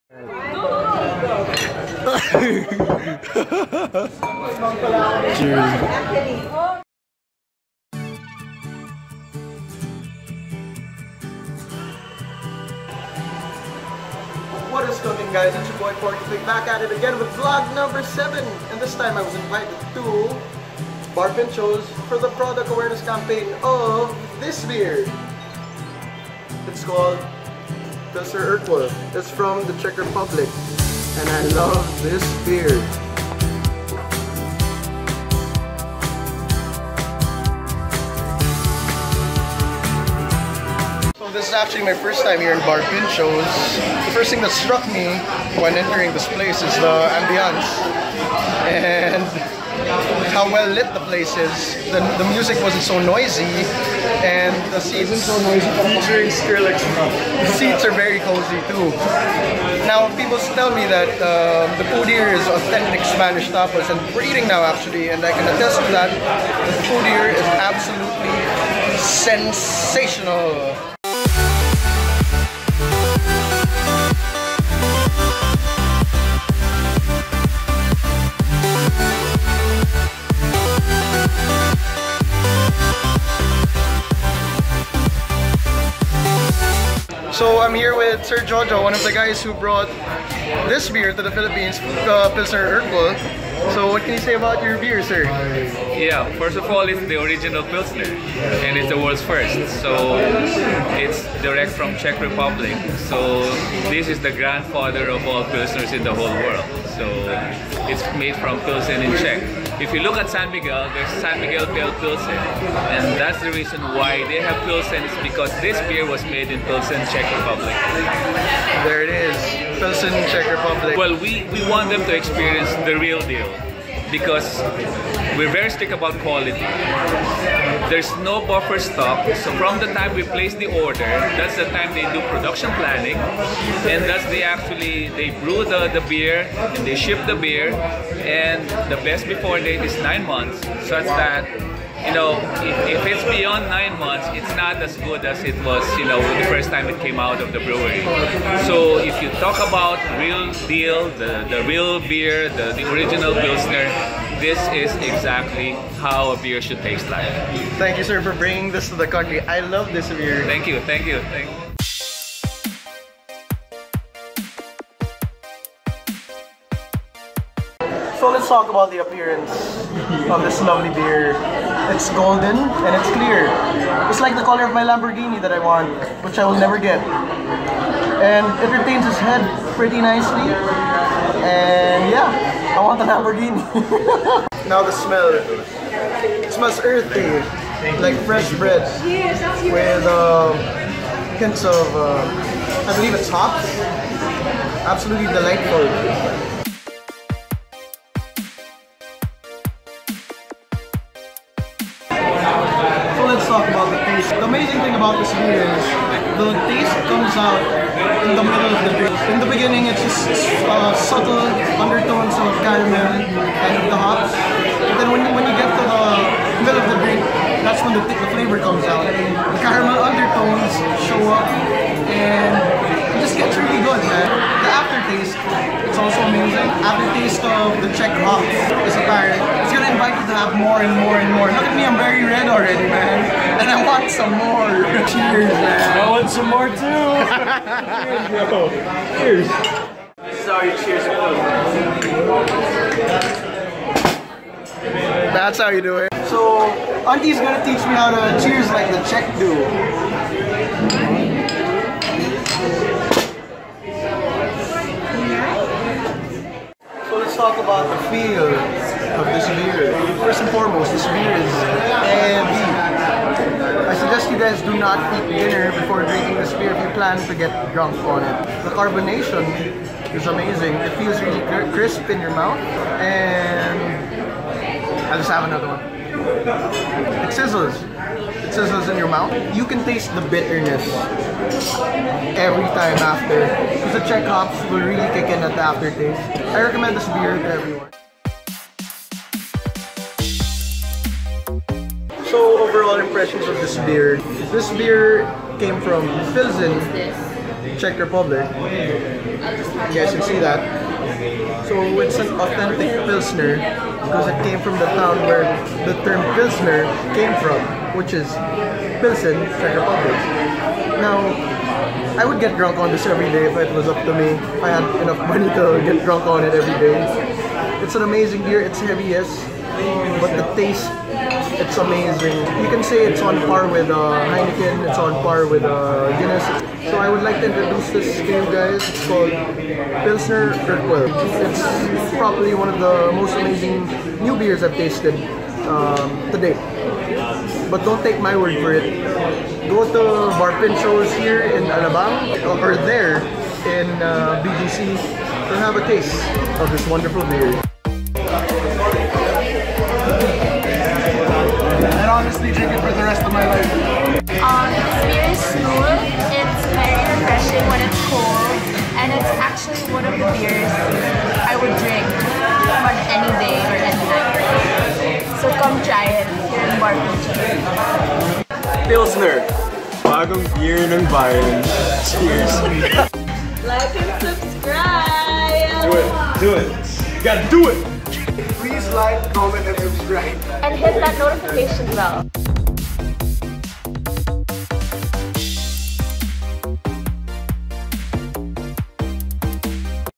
Pilsner Night @ Bar Pintxos Alabang | Porkupig Vlogs #7
What's cookin' guys? It's your boy, Porkupig!

This time, I was invited to vlog the product awareness campaign for Pilsner Urquell at Bar Pintxos Alabang. I would say that this beer tastes absolutely amazing; to the point that I couldn't count how many I had when I got home. The food in Bar Pintxos was sensational. Off the camera, I had good caviar and foie gras and the other food featured in the video.

Big big BIG thanks to Renzo, Anika, MC and Omi for being the wonderful people behind and in front of the camera. Big thanks also to auntie Beng and tito Jojo for giving me the opportunity to vlog their event.

Sorry for the bad audio quality. I do not own a lapel mic yet. I'm limited by the hardware that I own.

All filmed with a Samsung Galaxy S7 Edge.